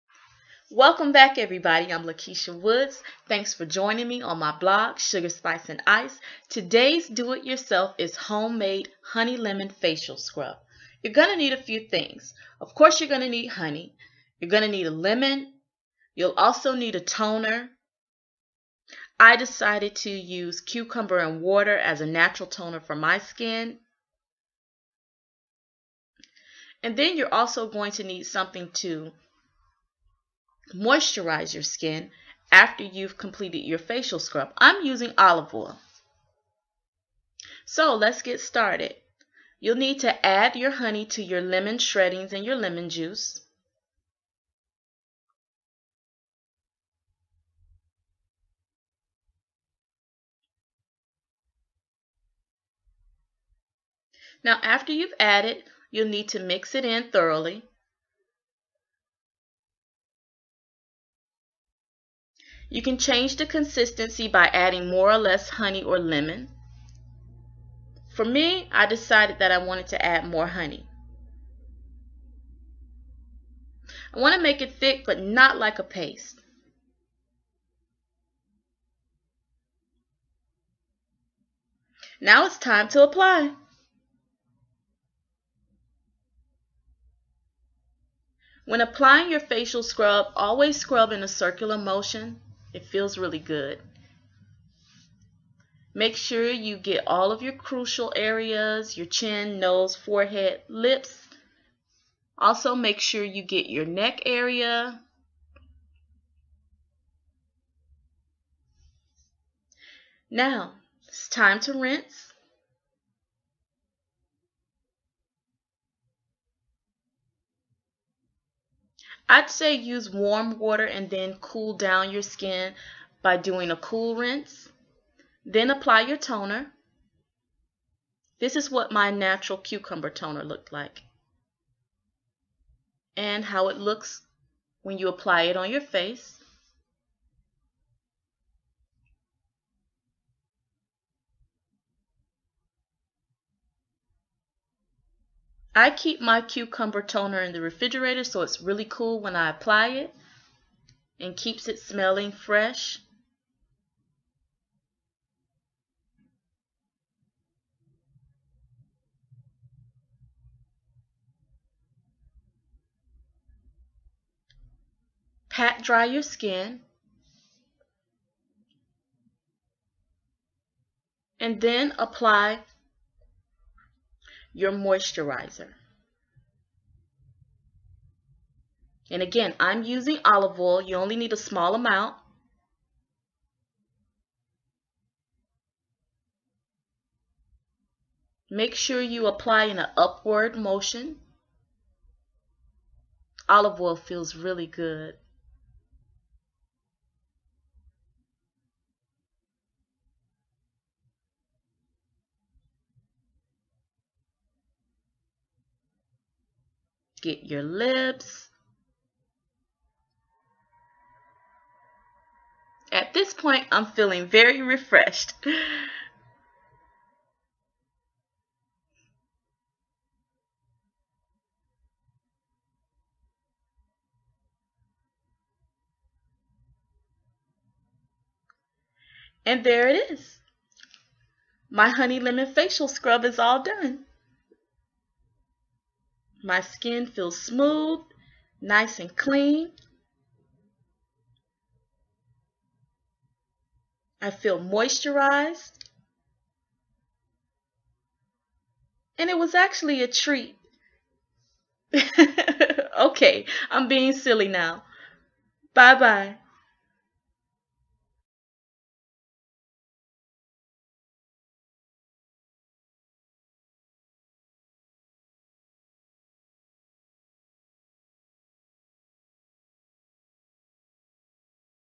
Lakesha Woods-Vblog #5 Honey Lemon Facial Scrub
Here's an easy "Do It Yourself" project - making honey lemon facial scrub. There's nothing better than being pampered. Going out to the spa to have someone do it for you is always a perk. Knowing how to do it yourself is surely a PLUS, especially if you're on a budget (like me). You've got to use what you got, UNTIL you get what you want! So why not DIY?!